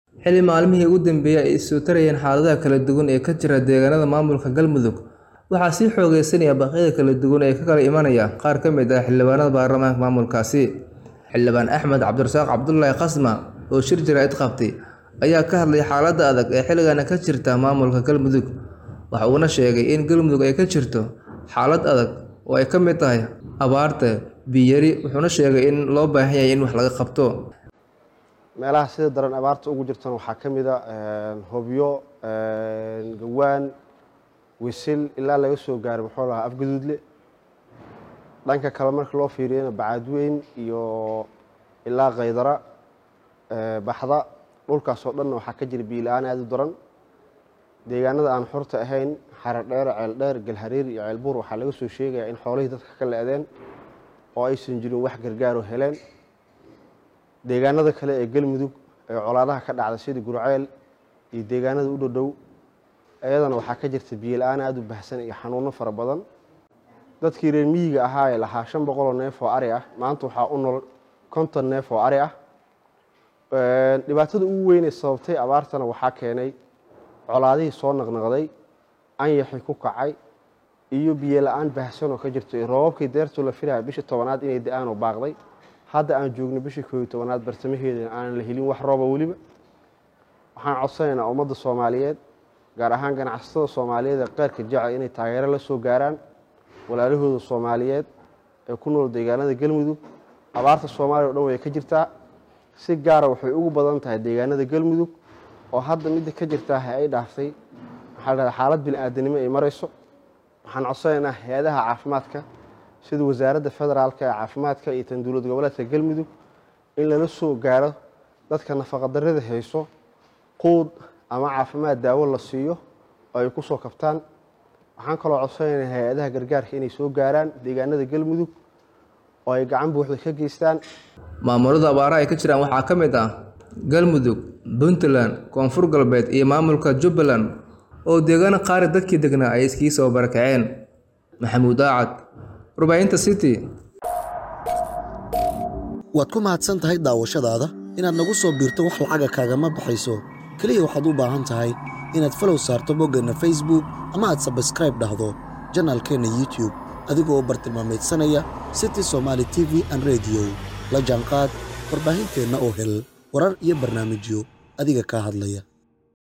Abaaro baahsan oo laga Soo Sheegayo Deegaano katirsan Maamulka Galmudug Maxamuud Dacad
Abaaro baahsan oo laga Soo Sheegayo Deegaano katirsan Maamulka Galmudug Maxamuud Dacad ayaa Kawaramaya